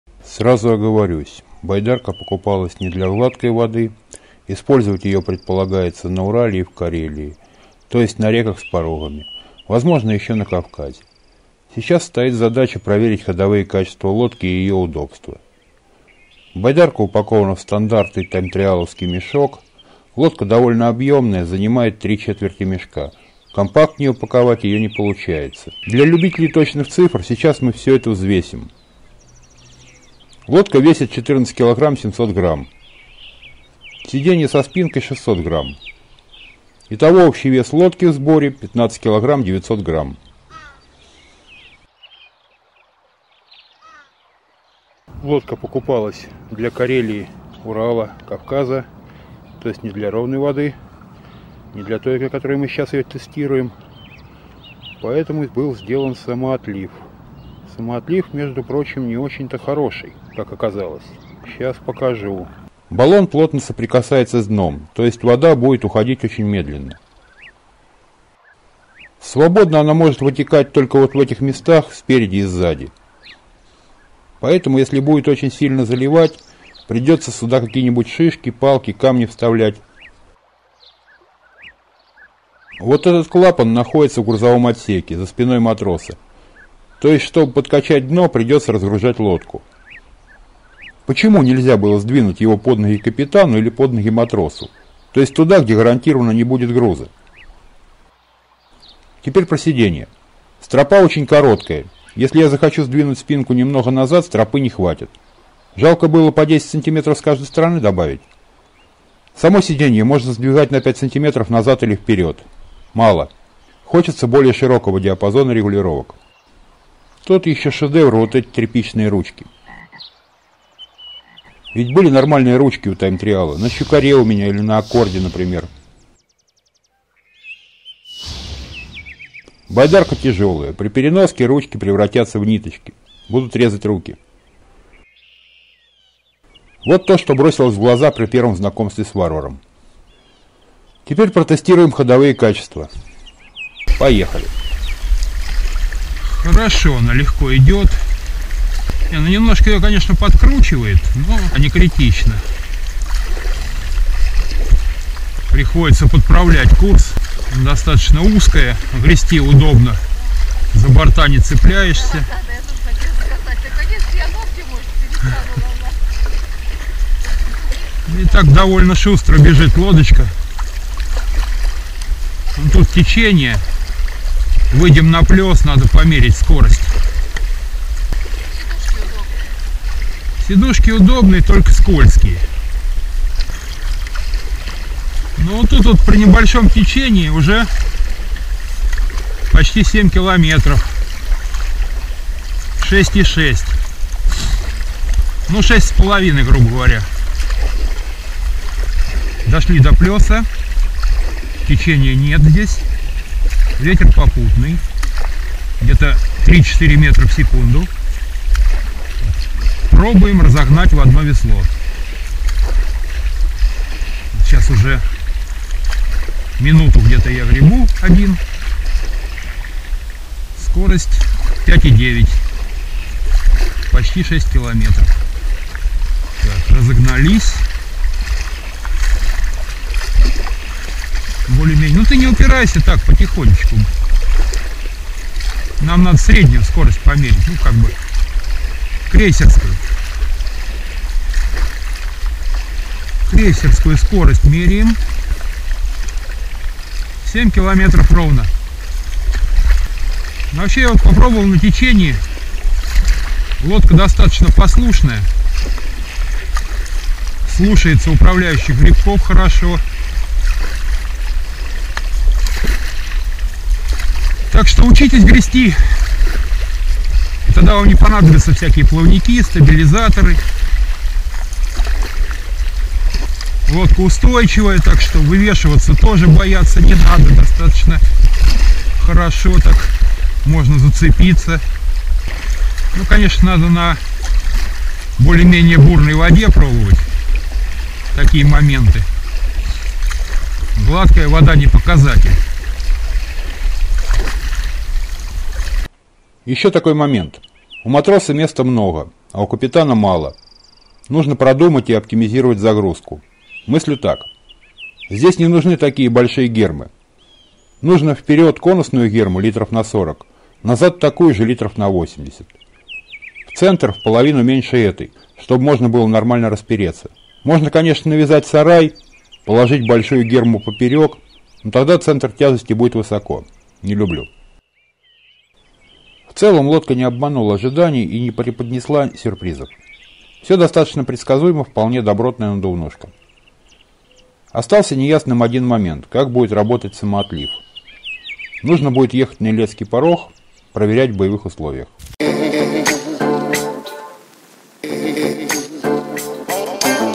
Настоящий тест-драйв надувного каяка «Варвар-480» от TimeTrial!
Надувная байдарка (каяк) "Варвар-480" — это усовершенствованное продолжение линейки надувных каяков "Варвар". Надувное, лёгкое, быстрое судно рассчитано на длительные плавпоходы по спокойным рекам и на экстремальные сплавы по бурным рекам и водопадам.

Элементарный в сборке и управлении "Варвар-480" отлично подойдет для автономных путешествий, в том числе одиночных.

За счет увеличенных габаритов, "Варвар-480" рекомендуется более крупным гребцам (ростом от 180 см). Каяк позволяет разместить больше груза и с комфортом совершать длительные путешествия.

Надувные элементы, создающие обвод обеспечивают Варвару высокую маневренность и гидродинамику (скорость хода). Он уверенно себя чувствует на речных просторах, прекрасно сохраняя баланс за счет курсовой устойчивости и изящной формы.

Приподнятые над водой корма и носовая часть дают Варвару по праву считаться одной из самых шустрых, манёвренных и мореходных лодок. Варвар с легкостью обходит водные препятствия, преодолевает волны, не зарываясь в воду.

Надувная лодка "Варвар-480" произведена по однослойной технологии (монобаллон), что облегчает ее сборку и ремонт. Бортовой баллон состоит из двух независимых надувных отсеков, разделенных мембранной перегородкой, для большей надежности и безопасности на воде.

Дно надувное, вклеенное (по бортам), состоит из 4 секций, где применяется запатентованная технология объемного шва. Состав (материал): высококачественная ПВХ ткань плотностью 750 г/м2.

Надувной каяк "Варвар-480" оснащен бедренными упорами и жесткой спинкой, позволяющими безопасно преодолевать самые сложные пороги. По бортам лодки расположены кольца, предназначенные для фиксации груза или обвязки. Ручки на носу и корме позволяют легко переносить лодку или закрепить чальный конец.

Выполнен надувной каяк "Варвар-480" в двухместном варианте, то есть установлено две сидушки на борту. Также следует отметить, что в данном случае места для вещей достаточно даже при посадке двух людей на борт. Двухместную лодку можно использовать как в одиночных сплавах, так и вдвоём. Удлинённые сверхпрочные полосы для крепления позволят перемещать сидение в удобное для Вас место.